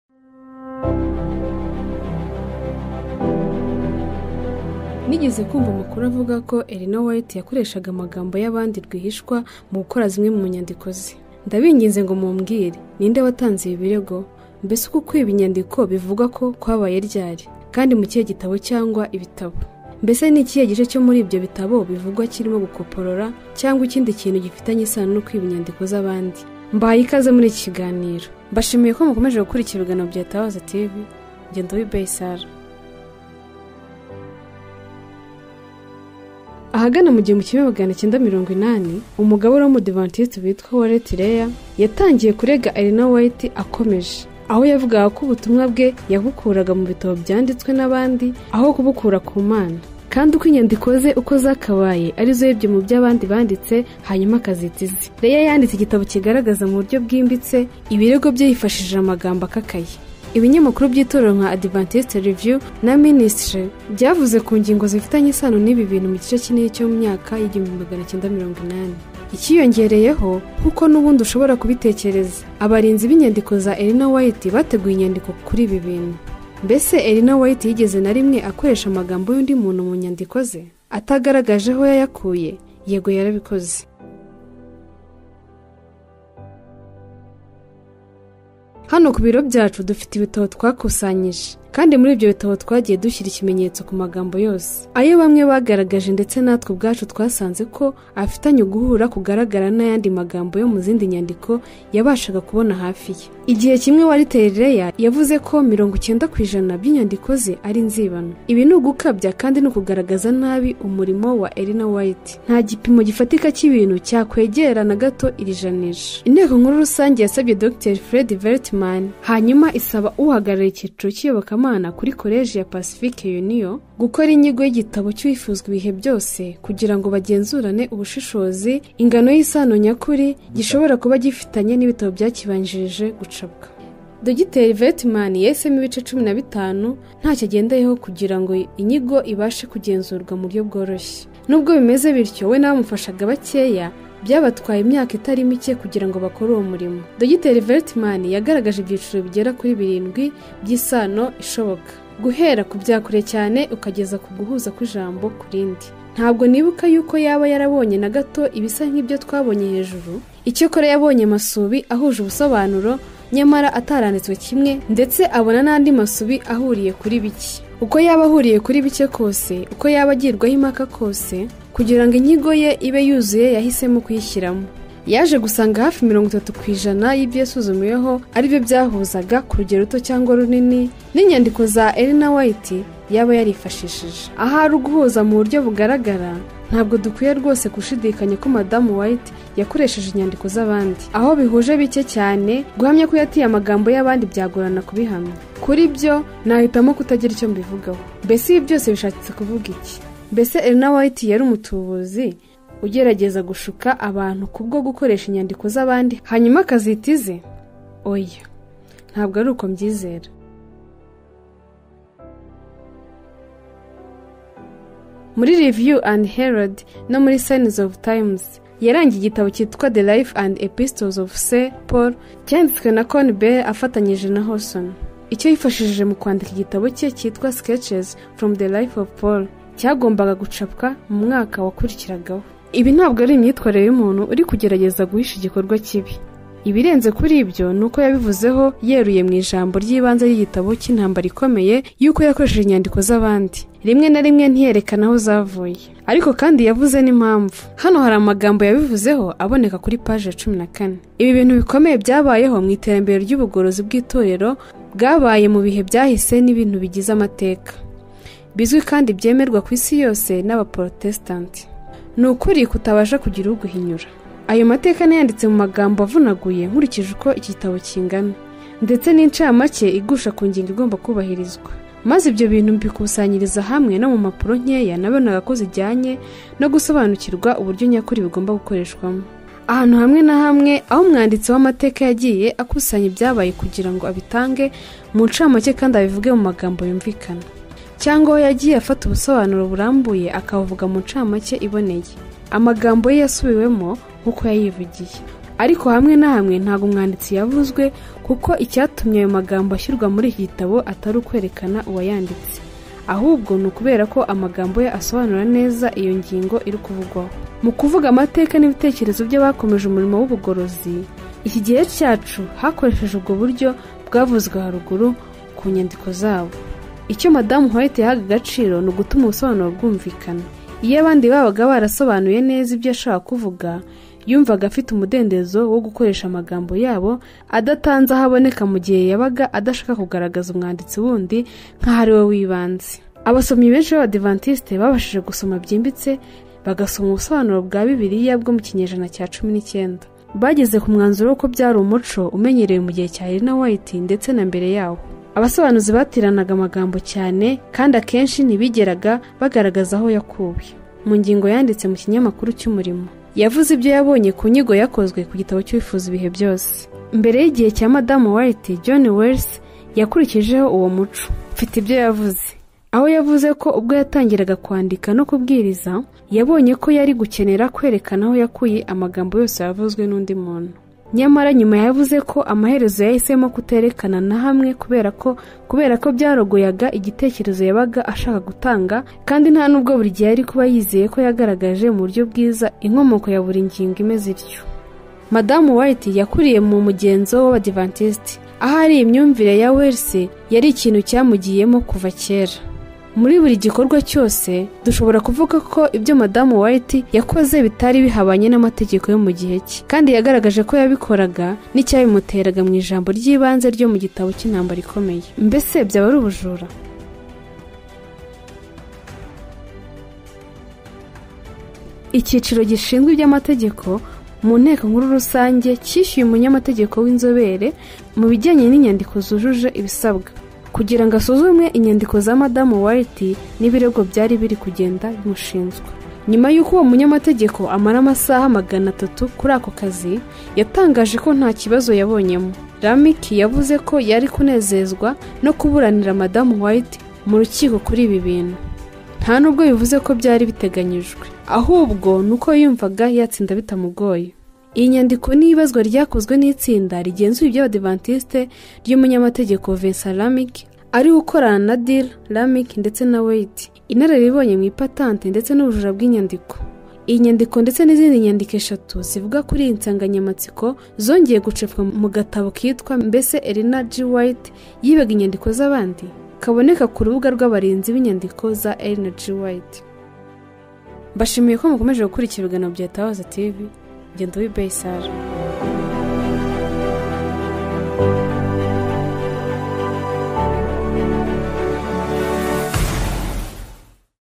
Ukuri kwirengagijwe: Ellen G. White ‘yibye inyandiko’ mu bitabo by’abandi azita ize?
Nigeze kumva amakuru avuga ko Elina White yakoreshaga amagambo y’abandi rwihishwa mu gukora zimwe mu nyandiko ze.

Ndabinginze ngo mumbwire, ninde watanze ibi birego? Mbese uku kwiba inyandiko bivugwa ko kwabaye ryari, kandi mu kihe gitabo cyangwa ibitabo?

ITABAZA is not responsible for the content of external sites.